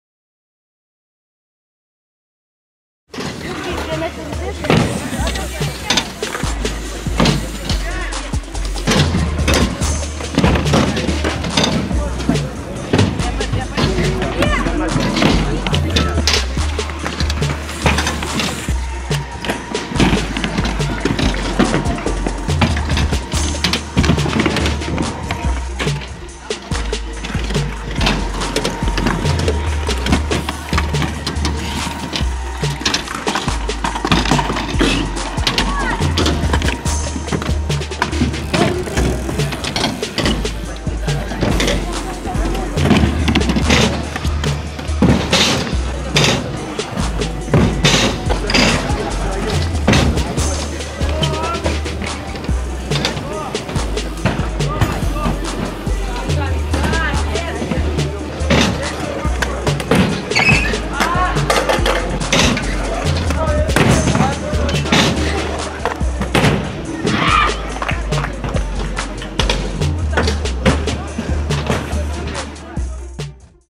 20150826 Максим Полуницкий, скейтпарк Кузьминки - пара трюков
это тот случай, когда мысли материализуются )) месяц назад заметил в парке Макса, подумал, что летает он неплохо и можно запилить эдит. Мы даже не договаривались, в какой день снимать, и вот )